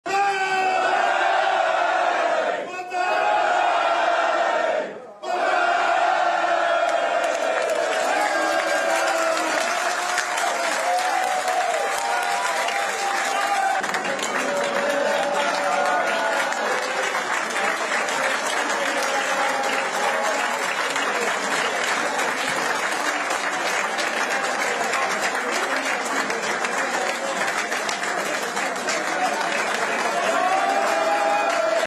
【参院選】芳賀道也氏（無所属：新）が山形で当選(19/07/21)
山形選挙区の芳賀道也氏（無所属：新）が当選。